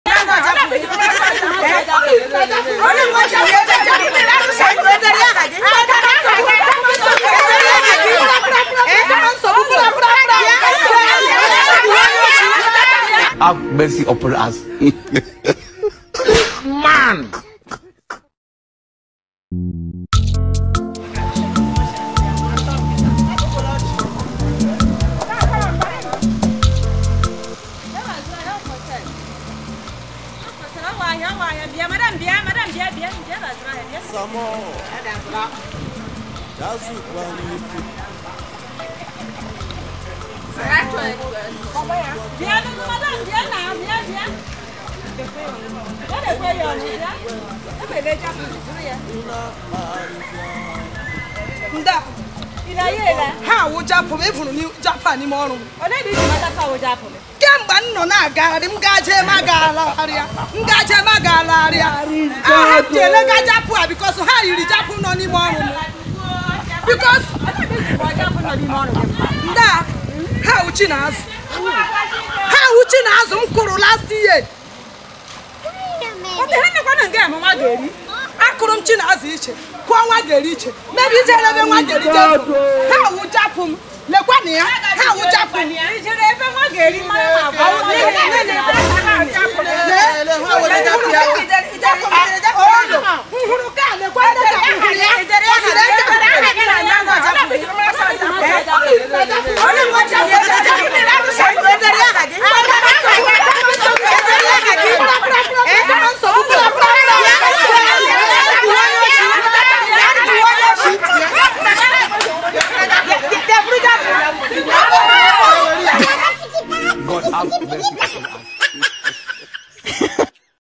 This is my Cassava@sir Moore Comedy Show
Jully told a woman selling cassava in the market that the cassava is her own. can you imagine that! watch to see how it all happened @sirmoorecomedyshow